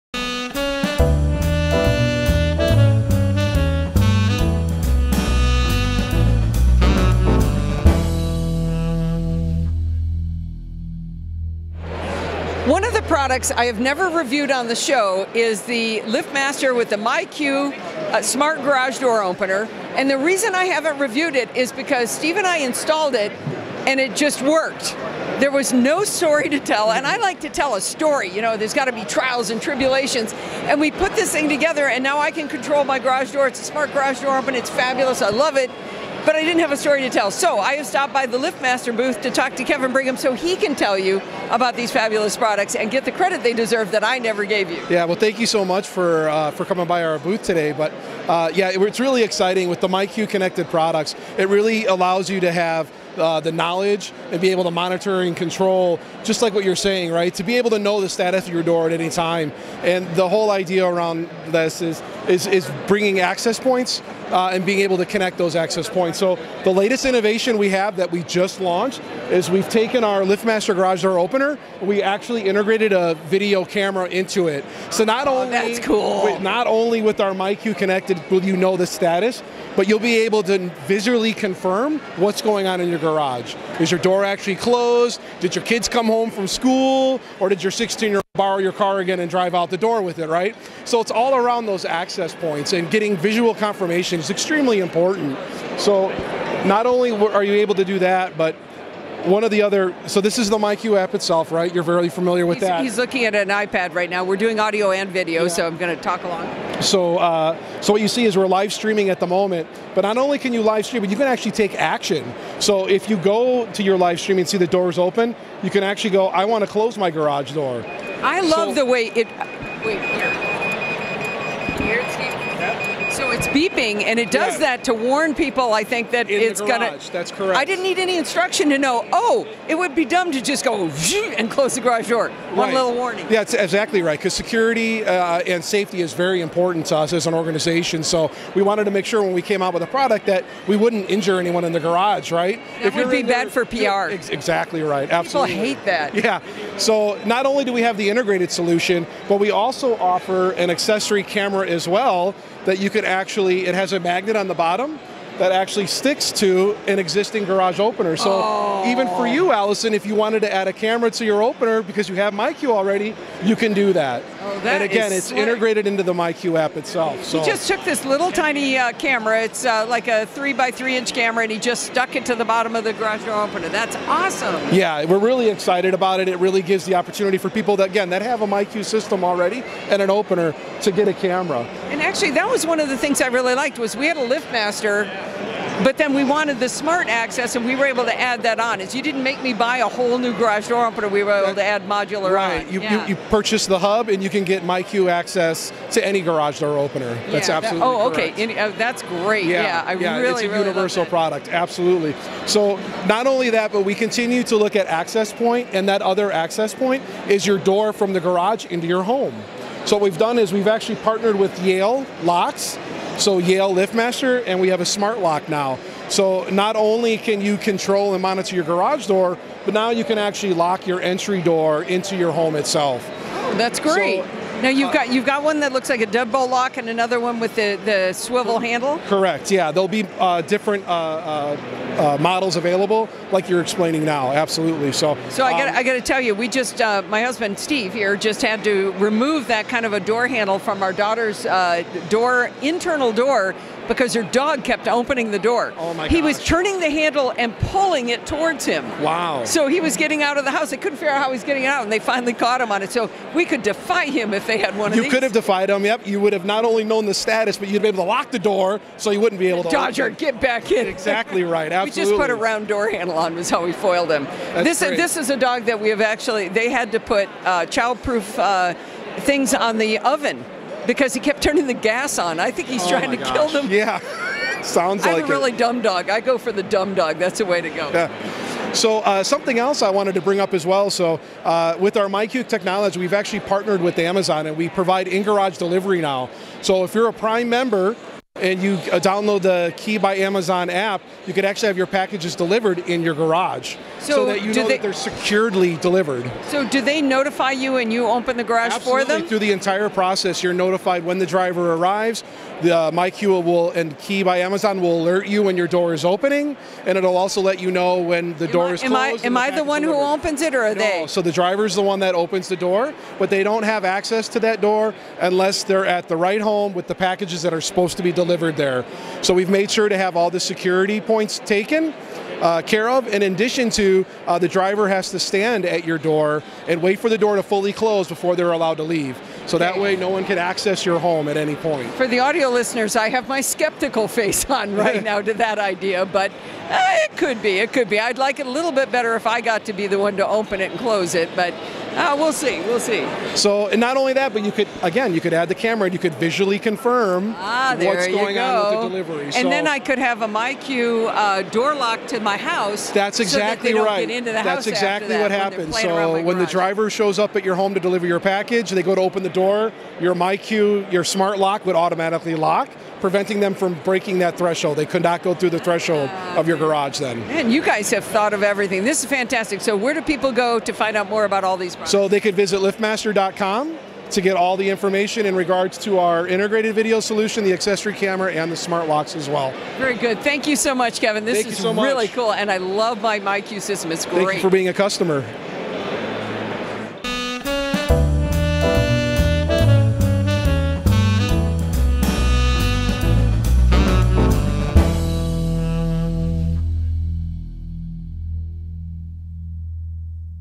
CES 2020: myQ Connected LiftMaster Smart Home Products
Allison Sheridan interviews Kevin Brigham from LiftMaster about several of their connected products. First up is the well-known LiftMaster garage door opener, but this version is updated with myQ technology. myQ connects your opener to your home WiFi and provides remote operation and monitoring. With myQ and its accompanying mobile app, you can receive activity alerts and open/close your garage door from your smartphone. You can also set recurring schedules to close your garage door and turn myQ lights on/off and you can sync with popular smart devices and platforms. You can buy LiftMaster garage door openers with myQ built in or you can upgrade your existing LiftMaster opener with myQ module. Kevin then shows the video camera that LiftMaster has integrated into their garage door opener. This camera allows you to monitor real-time video of your garage through the myQ app. Finally, LiftMaster has partnered with Yale to produce smart locks for your entryway doors. The setting is Pepcom's Digital Experience at CES at the Mirage Hotel in Las Vegas.